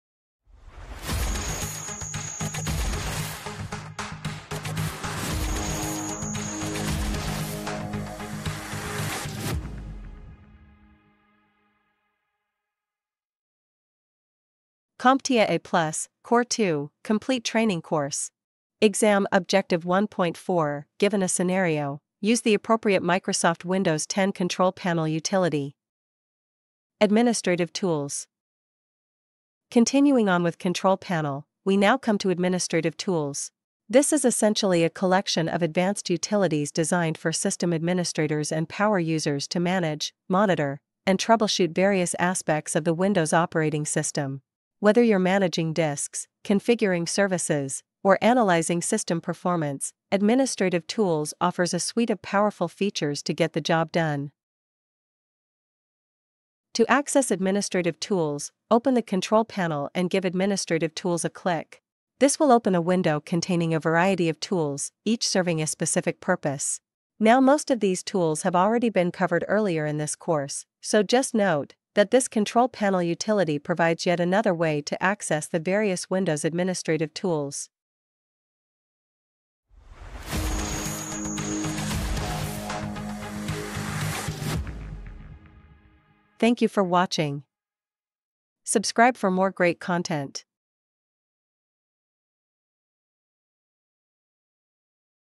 CompTIA A+ Core 2 (220-1102) | Administrative Tools | Exam Objective 1.4 | Course Training Video
CompTIA A+ Core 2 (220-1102) | Exam Objective 1.4 | Given a scenario, use the appropriate Microsoft Windows 10 Control Panel utility - Administrative Tools